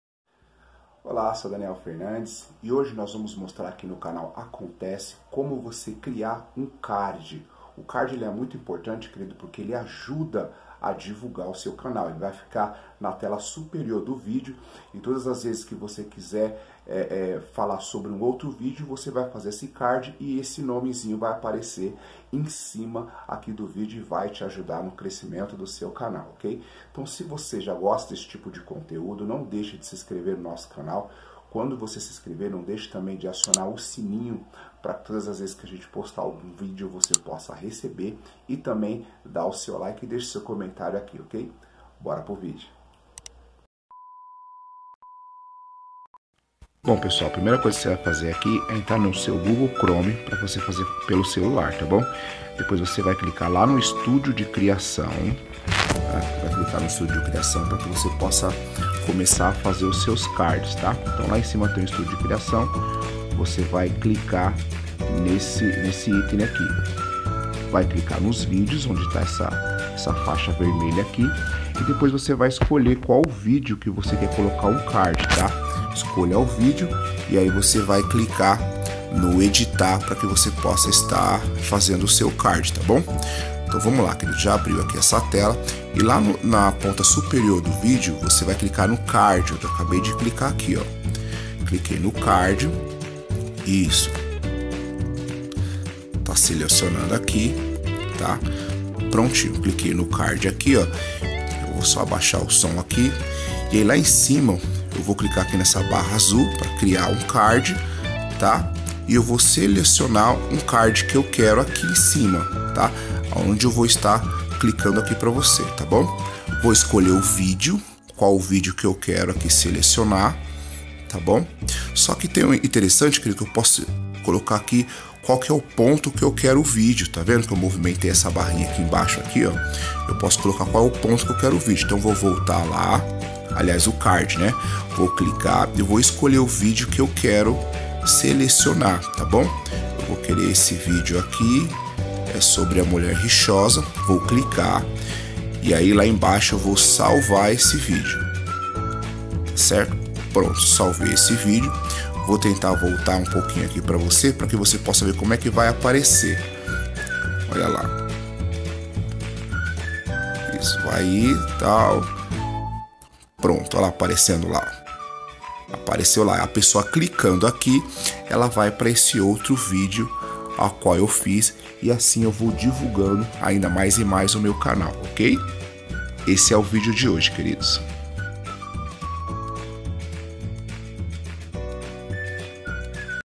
Como colocar CARDS no vídeo pelo celular
CANAL: Daniel Fernandes de tudo um pouco

No vídeo de hoje mostrarei como colocar CARDS no seu vídeo pelo celular.